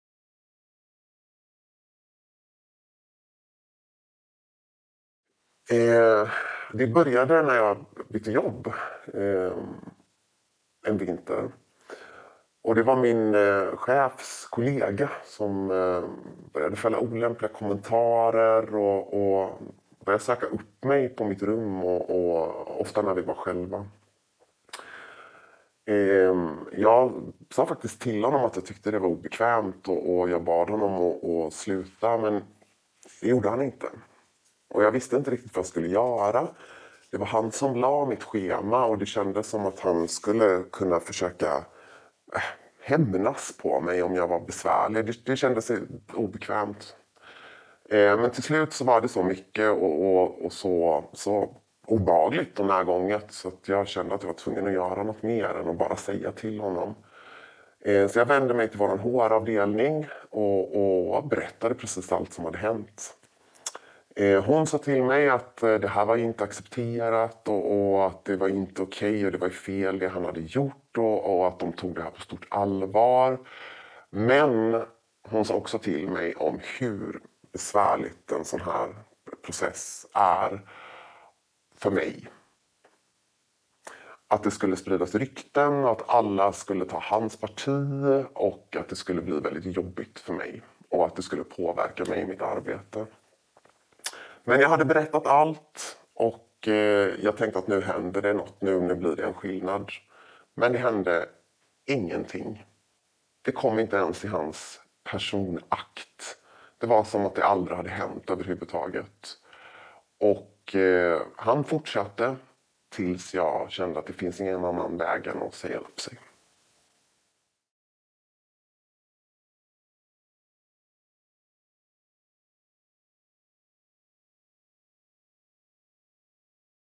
Sexuella trakasserier 2
Vi behöver ett bättre arbete mot sexuella trakasserier och diskriminering i Göteborgs stad!